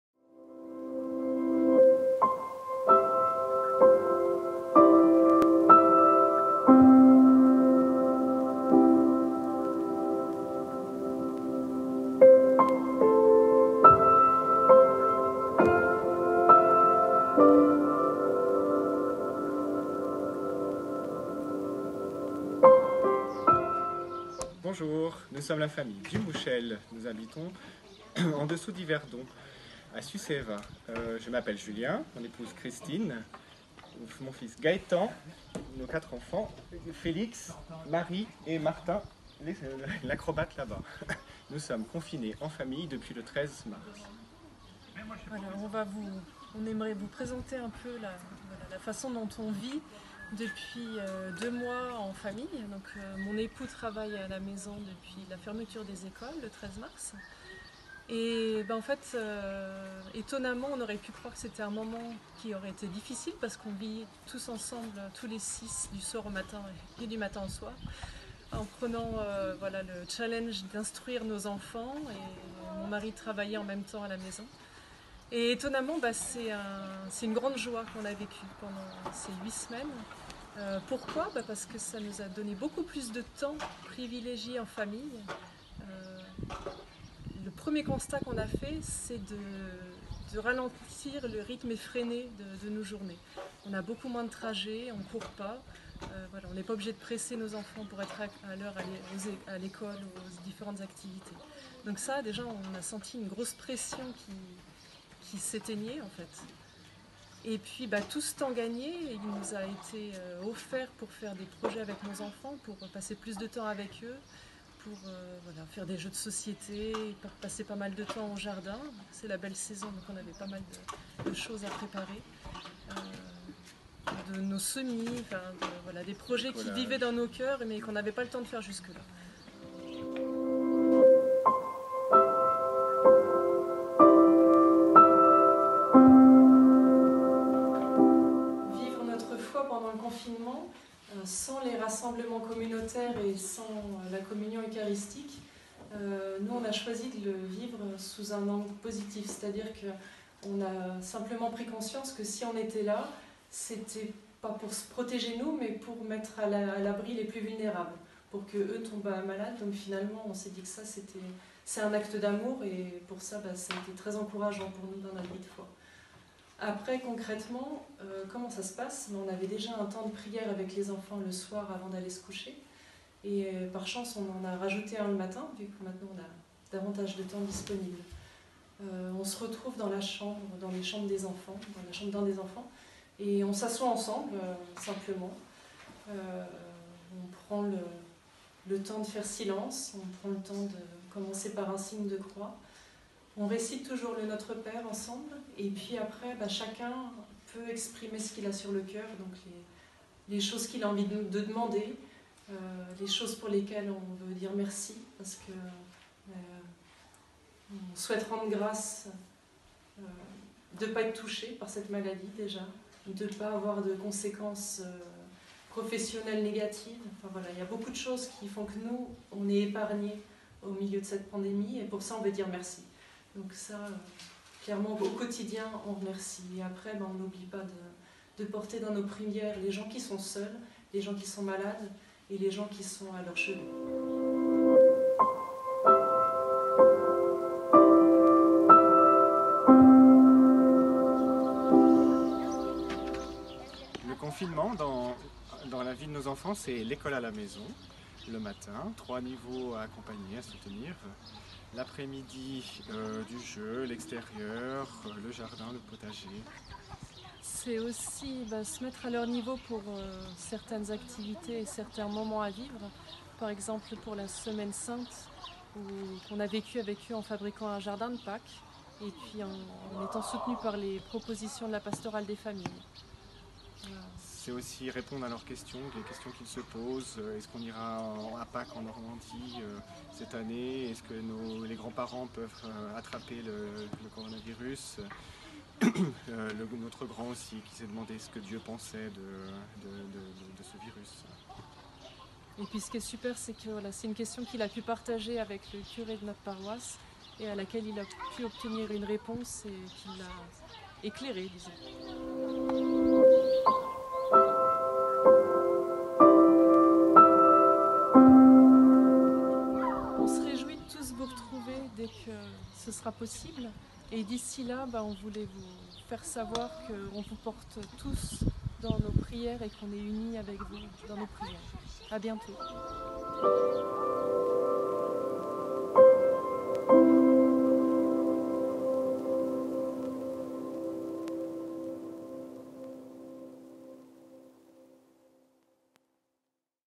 Confinement - tous d'un même coeur : épisode 2
L' Unité pastorale Chasseron-Lac propose chaque semaine un reportage vidéo sur le thème "Tous d'un même coeur". Reportage au sein d'une famille de 6 personnes qui raconte son quotidien et son expérience de foi en pleine pandémie.